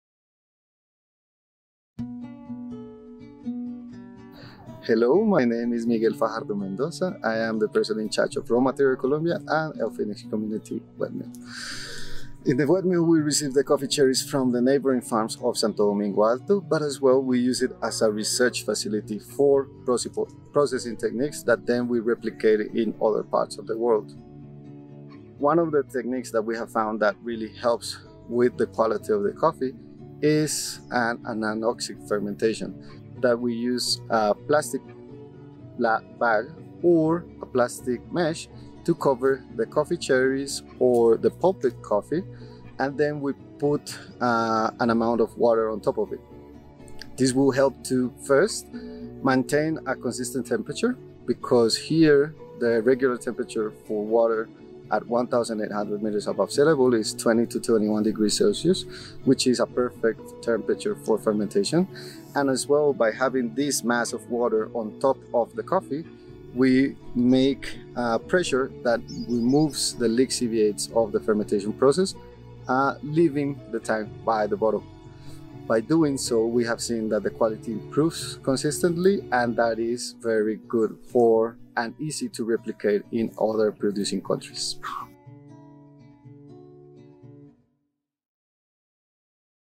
The Anoxic Water Pillow Technique at El Fénix
Miguel Fajardo Mendoza and Gilbert Mora demonstrate the anoxic water pillow technique. The process was created at the El Fénix Community Wet Mill, Colombia, and has tremendous capabilities for improving cup quality at low cost, for producers worldwide.